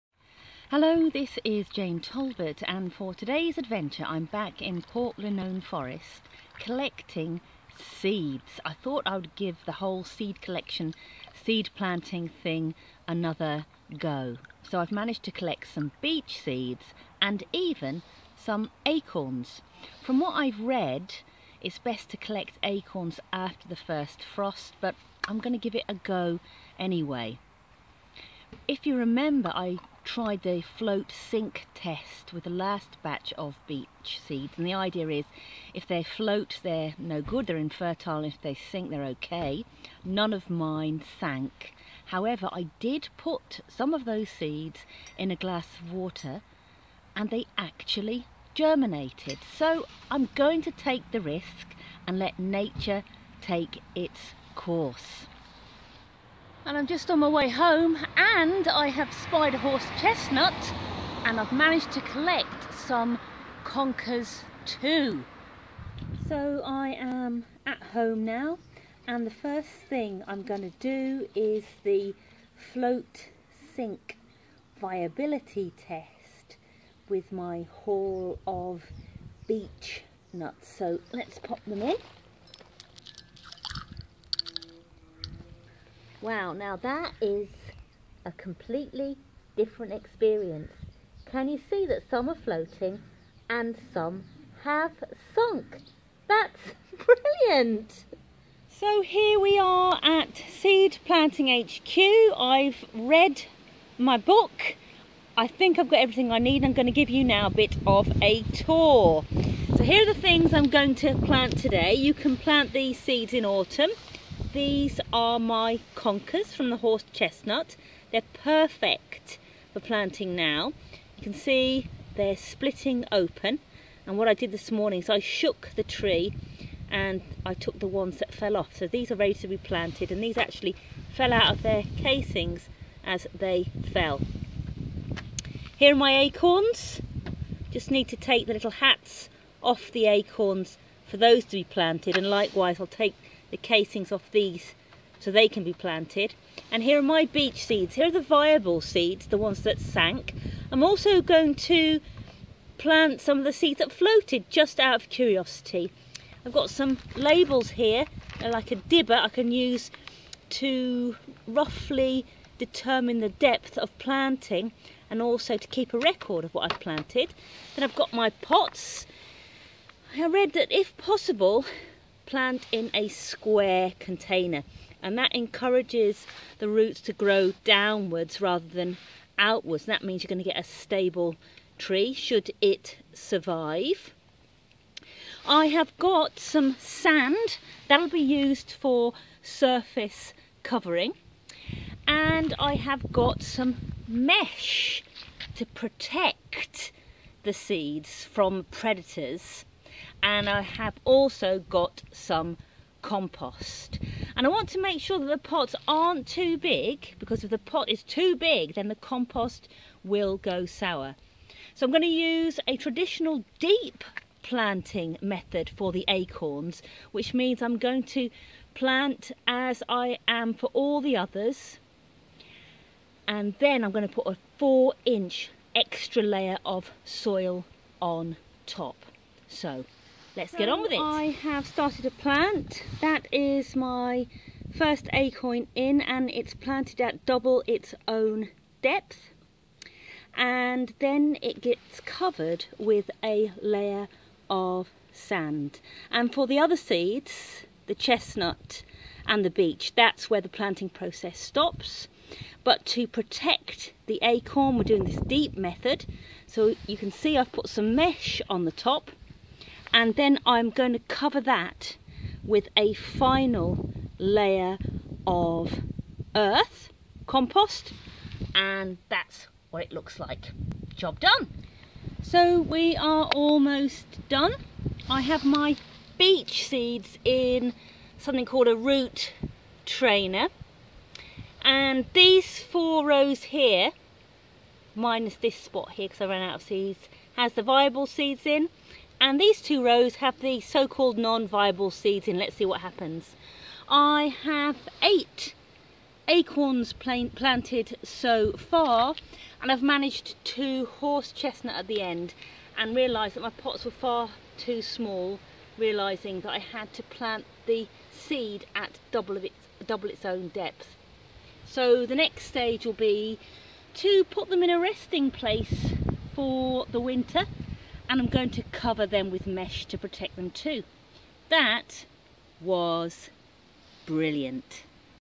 Collect And Plant Acorns, Beech Nuts And Conkers ~ Adventure 270 [365 Days of Adventure]
Today I collect tree seeds and plant them! [By the way - you can use the float-sink test with acorns and conkers too. And, I forgot to mention that the extra compost layer and "hidden" gauze marker that I use for the acorns can be removed in Spring!)

Jane Talbot